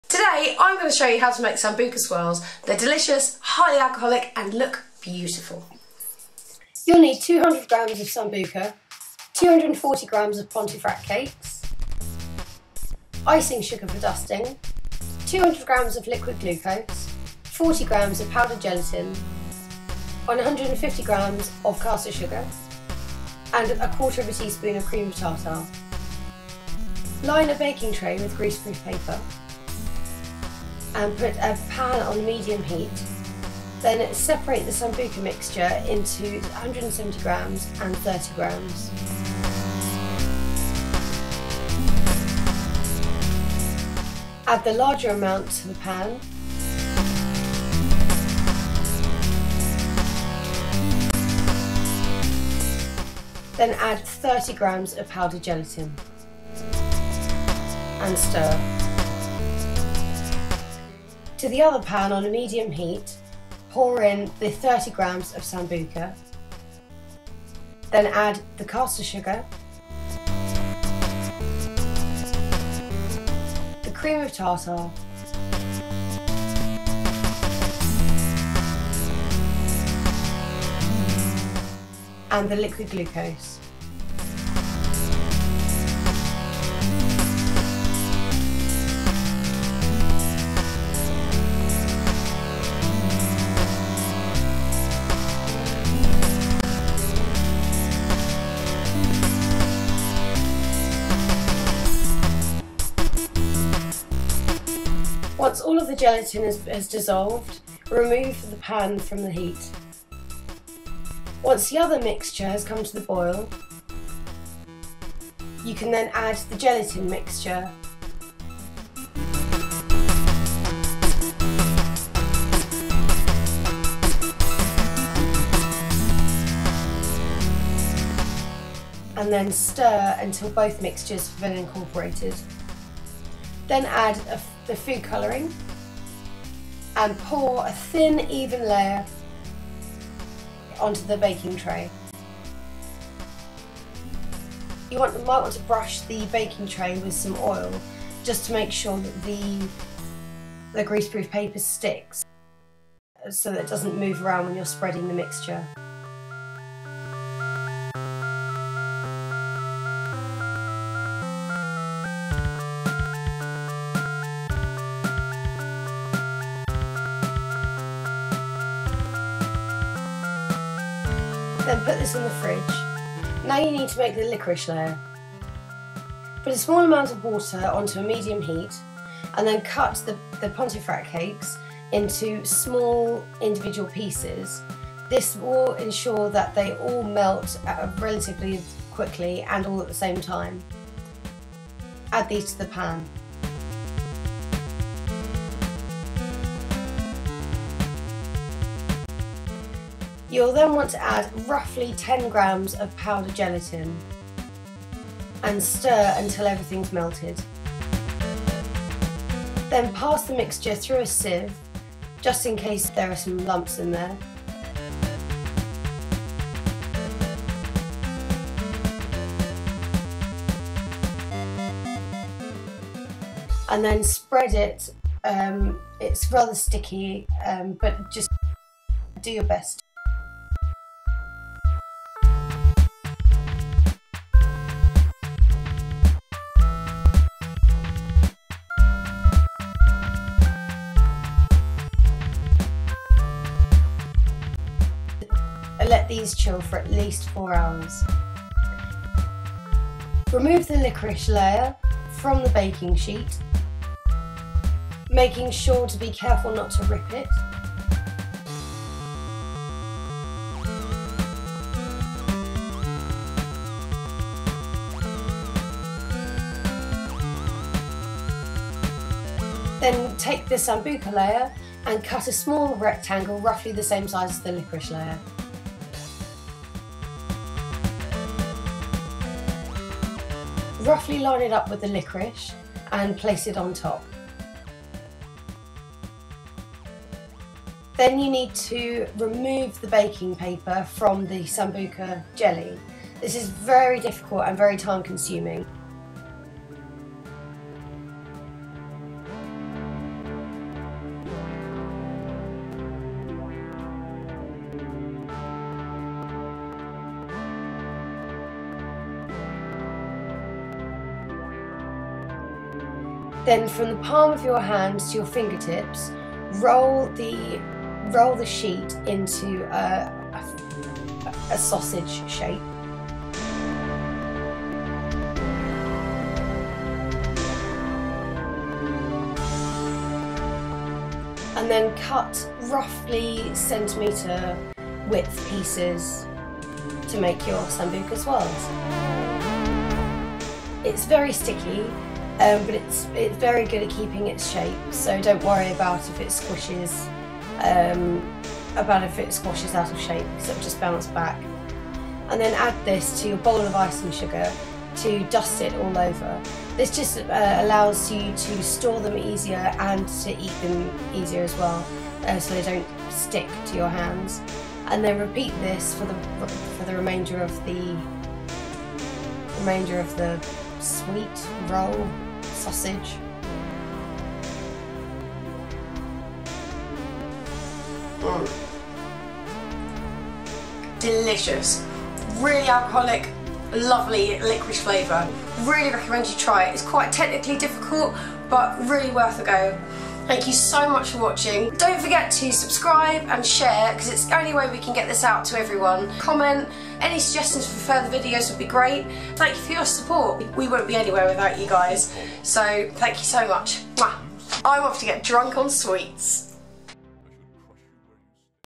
How to make Sambuca Swirls (Alcoholic Candy)
Milly shows you how to make the delicious and alcoholic treat; the Sambuca swirl.

Ingredients:
Sambuca 200g
Pontefract Cakes 200g
Icing Sugar
Liquid Glucose 200g
Powdered Gelatine  40g
Caster Sugar 150g
Cream of tartar 1/4 tsp